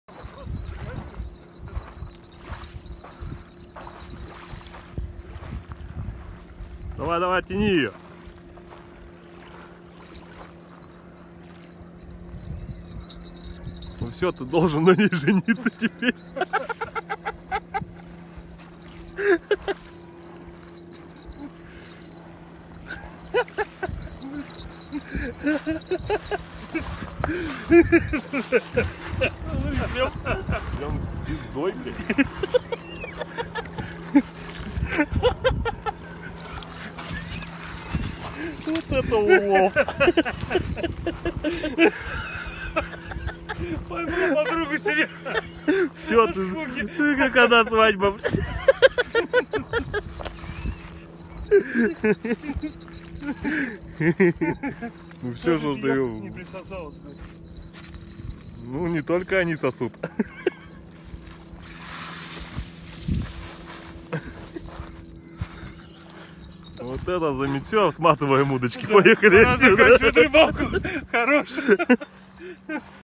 Поймал утопленника на удочку
Страшный случай на рыбалке